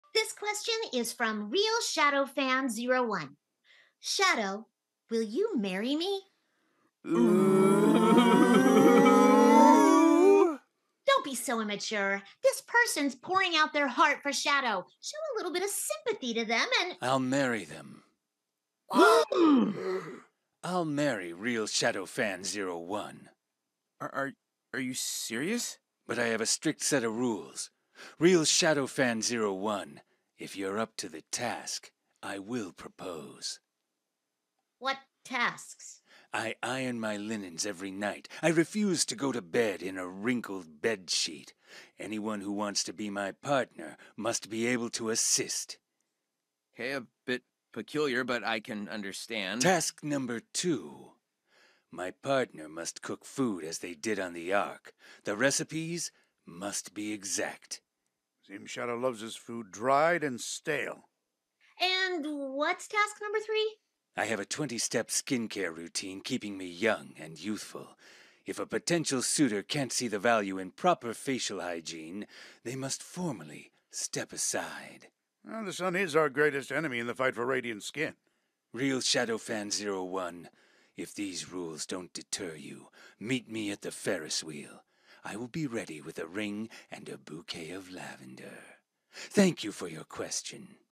finished realshadowfan01’s proposal
this has been in my capcut drafts for months but i finally finished it, it’s very lazy because i can’t draw them so pictures from google have to do